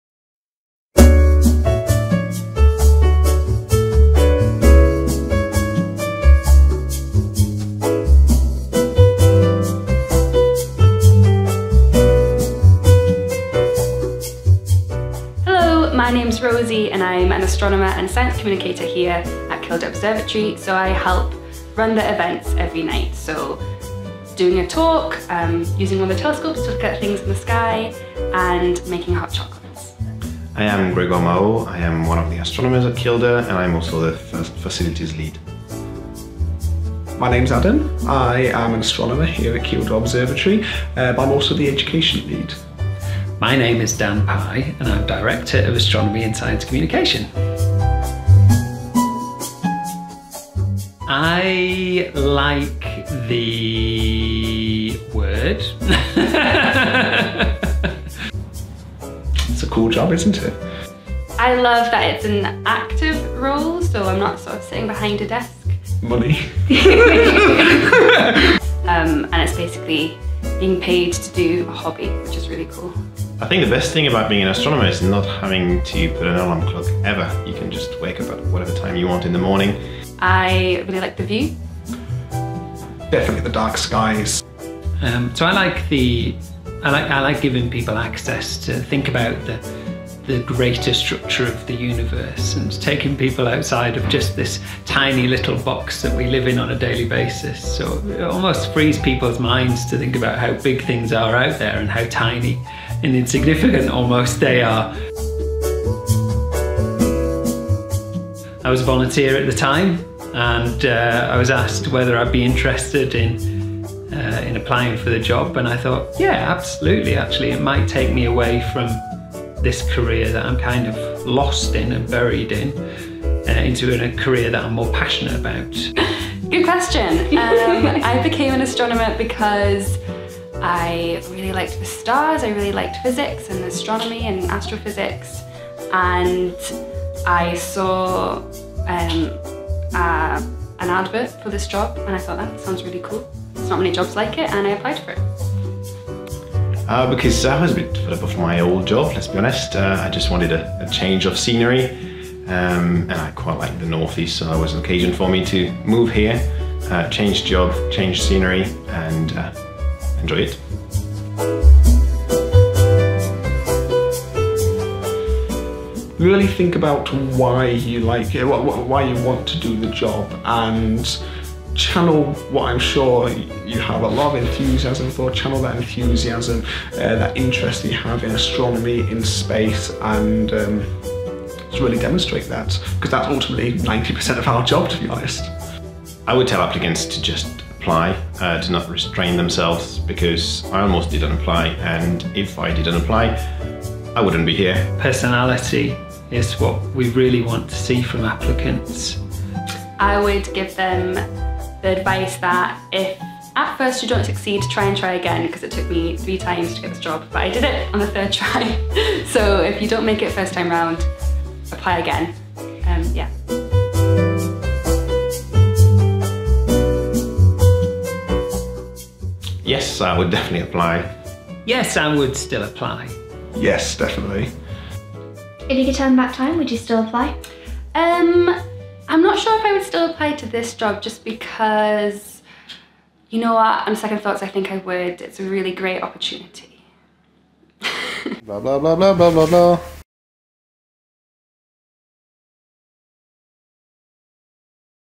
What does an astronomer do at Kielder Observatory?
As you may be aware, we're hiring!

Our Astronomer Ellie caught up with some of the team to ask them questions about being Astronomers, why they applied, and to provide advice to any hopeful applicants.

No Astronomers were harmed in the making of this video. Much.

For more information about the role and how to apply, scan the QR code at the end of the video or go